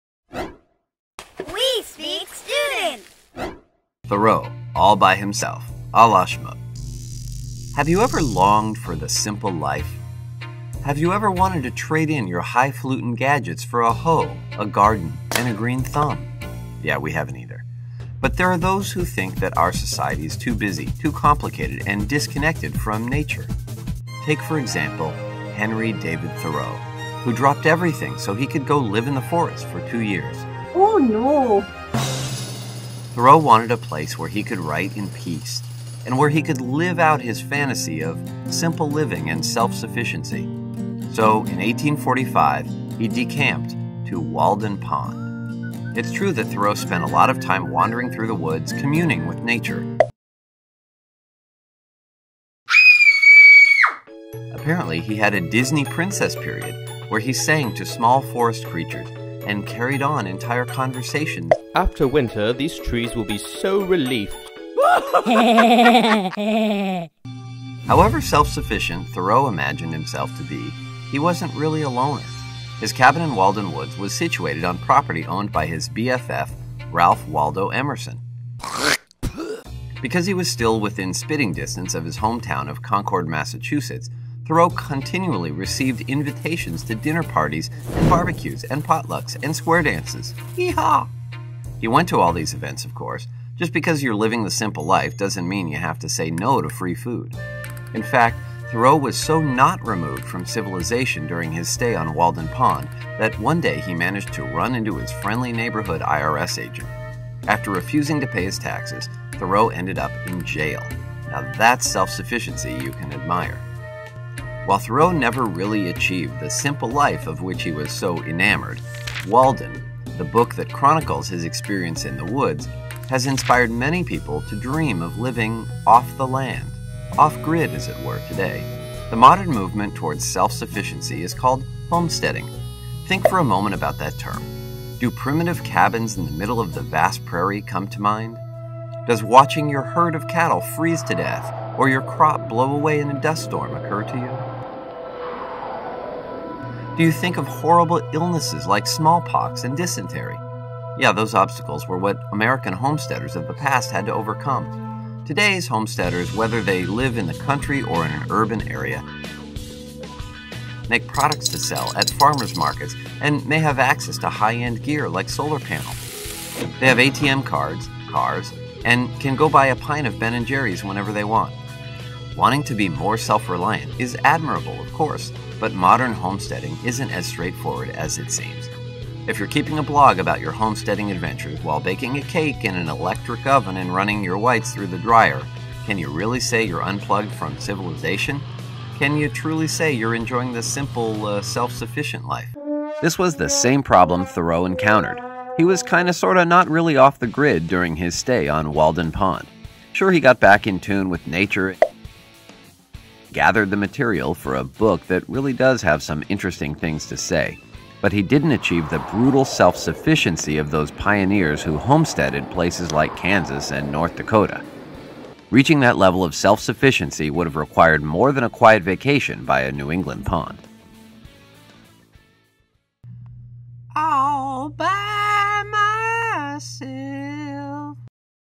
Thoreau: All By Himself by Shmoop
We've always wanted to go rogue and live au natural in the forest, but then we realize that conversing with trees and rocks can get pretty one-sided.